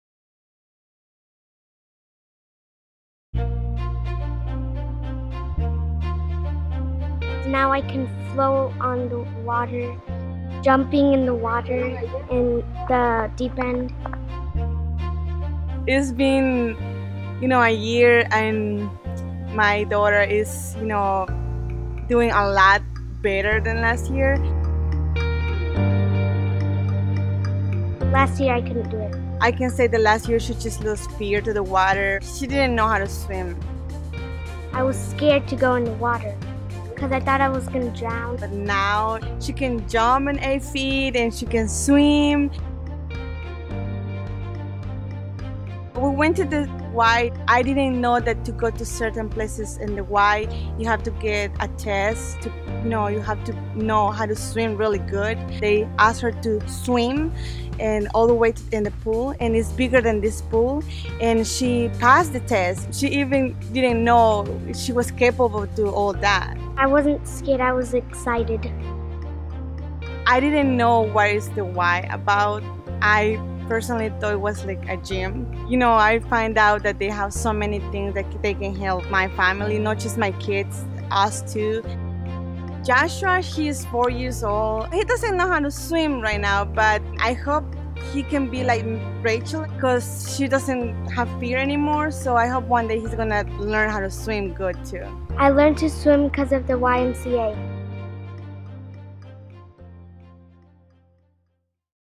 Vanessa's Y Story: Part 2
You may remember Vanessa. Last year, she brought her daughter Rachel to the Y’s Safety Around Water program at her local apartment community. Rachel was fearful of the water and unable to swim. Now, after being a part of the program for two summers, she is beaming with confidence, jumping in the deep end and swimming like a fish. Vanessa has even started bringing her kids to the YMCA, where Rachel recently passed her swimming skills test and earned her green brand.